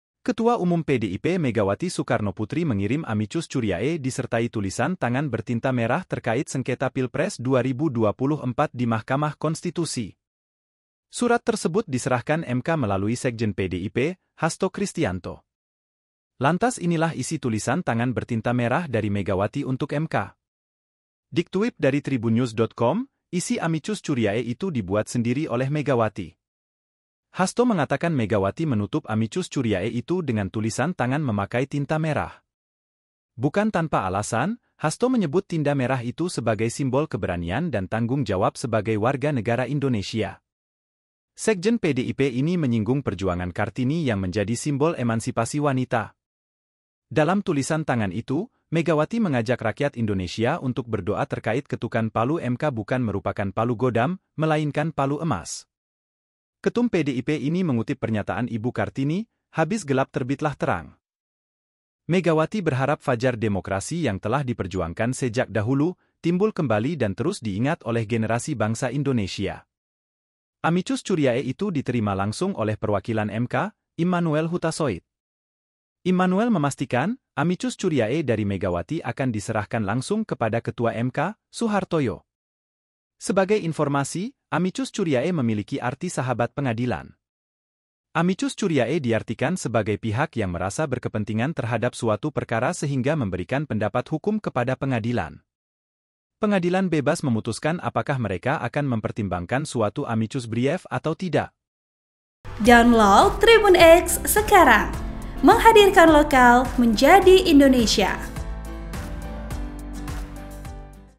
BERI PESAN MENDALAM, Megawati Titip Surat Tulisan Tangan Bertinta Merah ke MK dalam Amicus Curiae
VP: ARH

SURYA.CO.ID - Ketua Umum PDIP Megawati Soekarnoputri mengirim amicus curiae disertai tulisann tangan bertinta merah terkait sengketa Pilpres 2024 di Mahkamah Konstitusi.

Surat tersebut diserahkan MK melalui Sekjen PDIP, Hasto Kristiyanto.

Lantas inilah isi tulisan tangan bertinta merah dari Megawati untuk MK.

Diktuip dari Tribunnews.com, isi amicus curiae itu dibuat sendiri oleh Megawati.

Hasto mengatakan Megawati menutup amicus curiae itu dengan tulisan tangan memakai tinta merah.

Bukan tanpa alasan, Hasto menyebut tinda merah itu sebagai simbol keberanian dan tanggung jawab sebagai warga negara Indonesia.

Sekjen PDIP ini menyinggung perjuangan Kartini yang menjadi simbol emansipasi wanita.

Artikel ini telah tayang di Tribunnews.com dengan judul Isi Amicus Curiae yang Dikirimkan Megawati ke MK, Ditutup Tulisan Tangan Bertinta Merah,
Penulis: Jayanti TriUtami
Editor: Nanda Lusiana Saputri